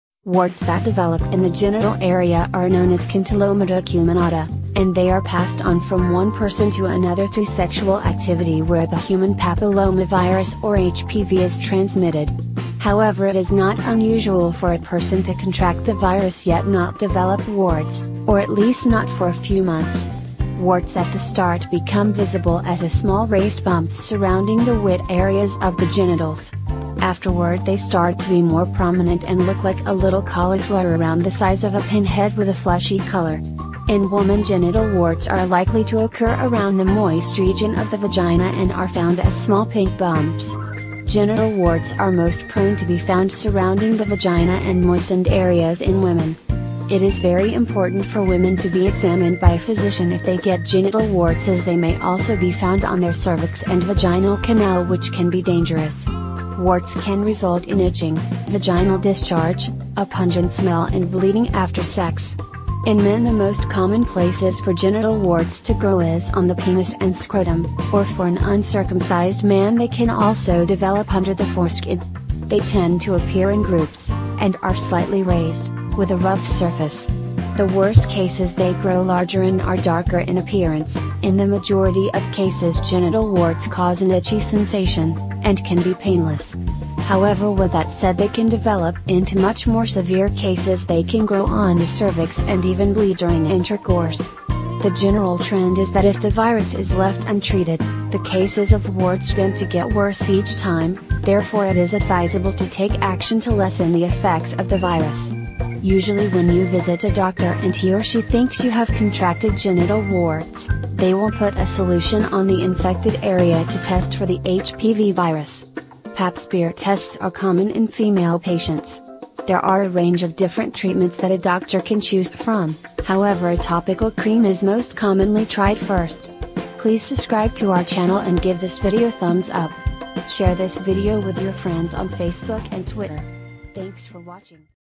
What is Molluscum Contagiosum and Warts? Signs, Symptoms and Pictures
Do not self-diagnose, visit your doctor for more detailed information.